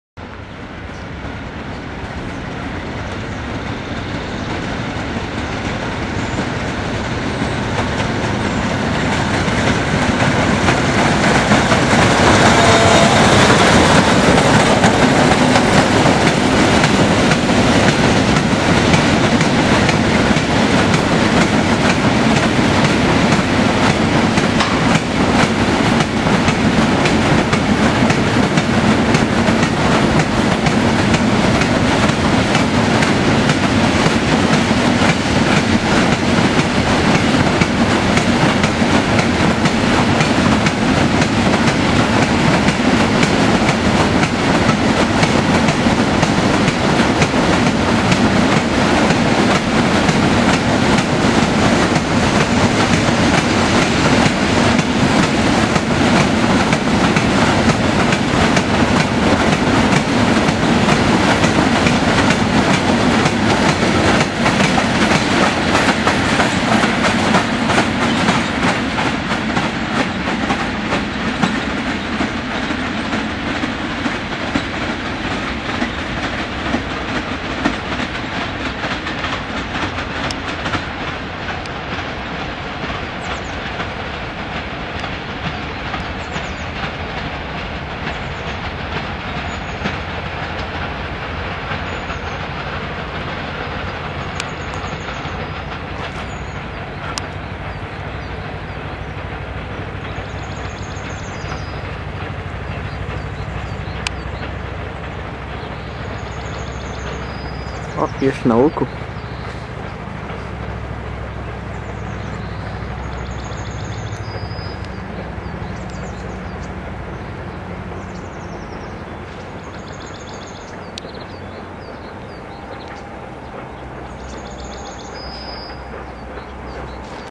ST43-265 w Bogaczewie
4 kwietnia2009.ST43-265 jedzie przez Bogaczewo ze skladem towarowym(chyba szutrowkami)